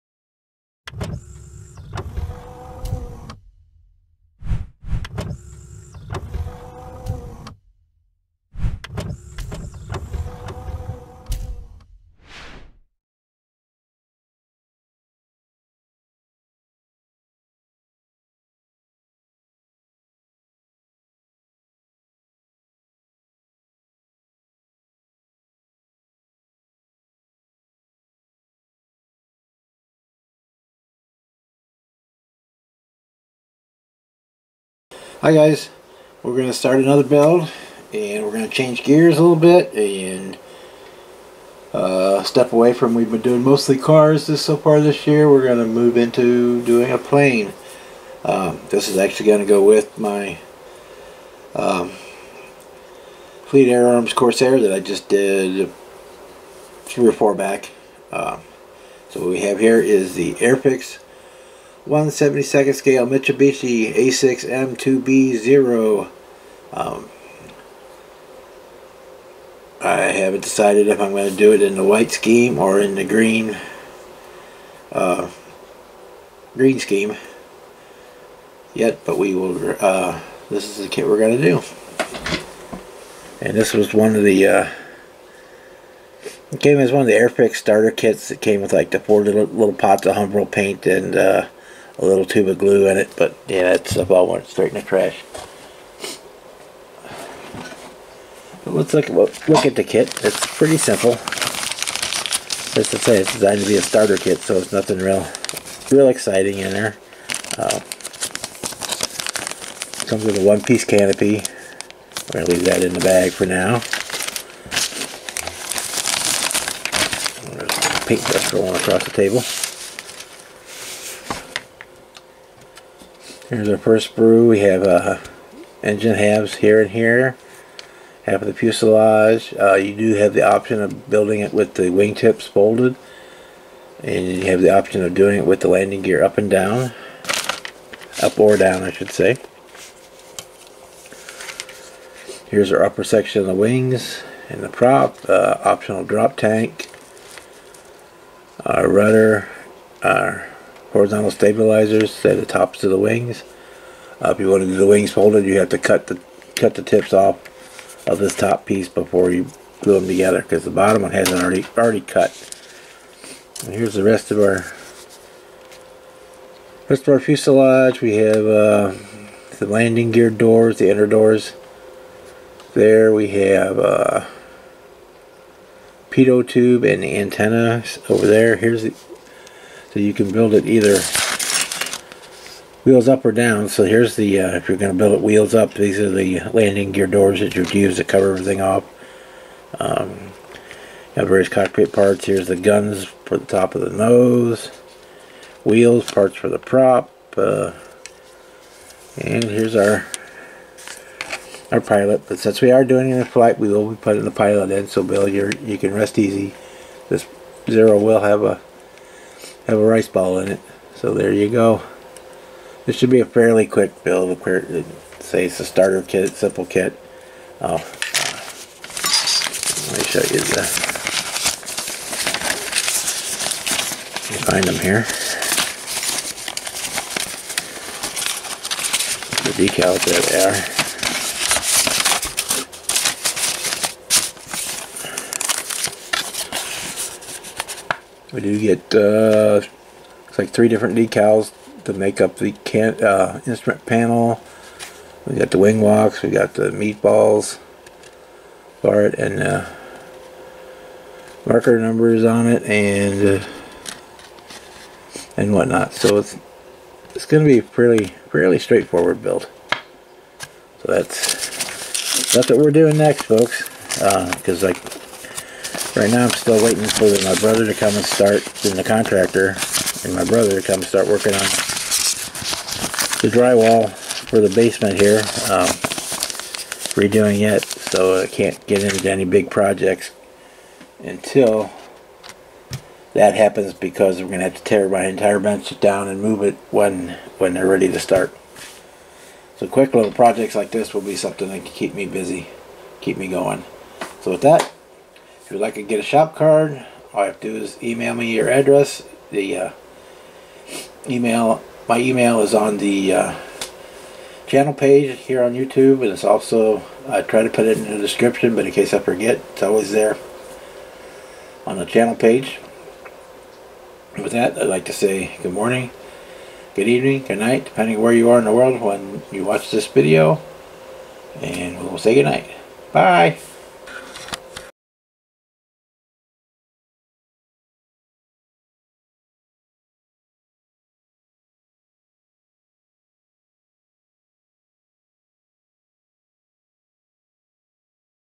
Airfix Zero intro
Airfix 1/72 scale Mitsubishi A6M2b Zero
Flown by Lt. Saburo Shindo
lead aircraft Carrier Division 1 Air Superiority Command
2nd strike unit IJN Carrier Akagi
Pearl Harbor 7 December, 1941

or a shop card, email your address to